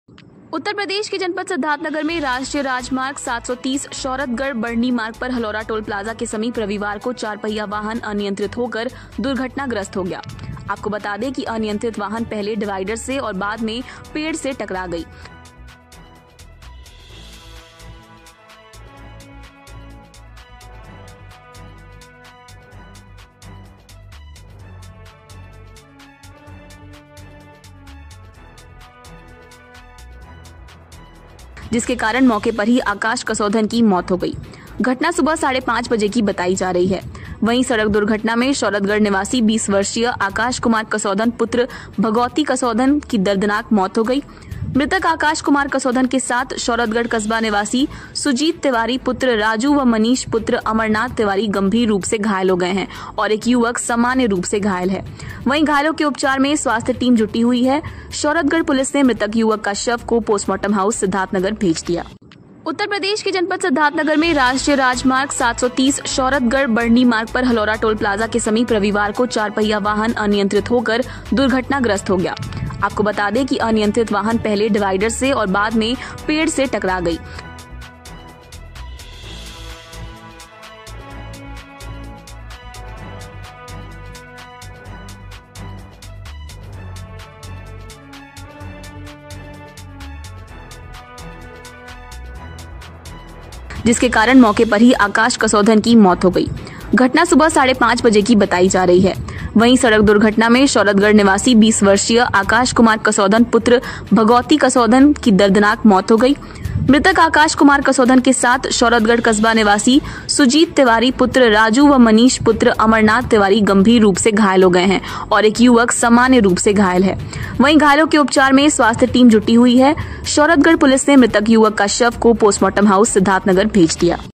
सिद्धार्थ नगर एनएच-730 शोहरतगढ़-बढ़नी मार्ग पर चार पहिया वाहन अनियन्त्रित होकर हुई दुर्घटनाग्रस्त
#uttarpradesh breaking up ft सिद्धार्थ नगर एनएच-730 शोहरतगढ़-बढ़नी मार्ग पर चार पहिया वाहन अनियन्त्रित होकर हुई दुर्घटनाग्रस्त